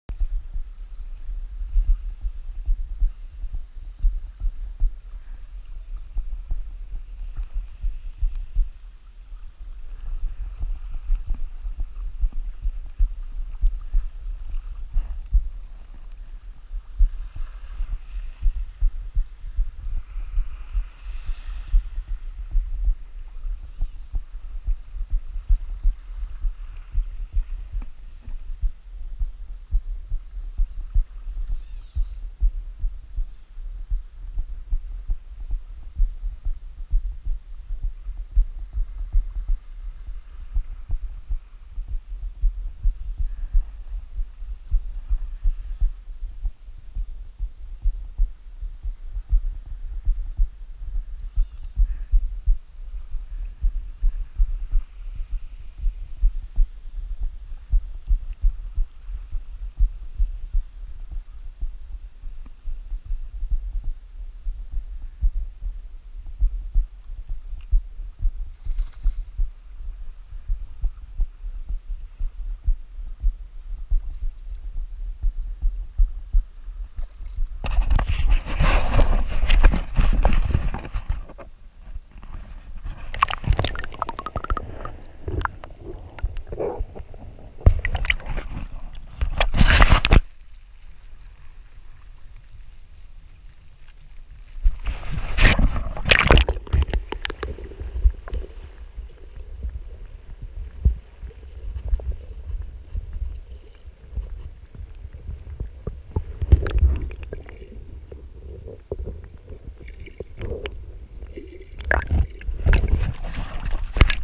Seychelles Alphonse island Bonefish
Another beautiful bonefish comes to hand on Alphonse island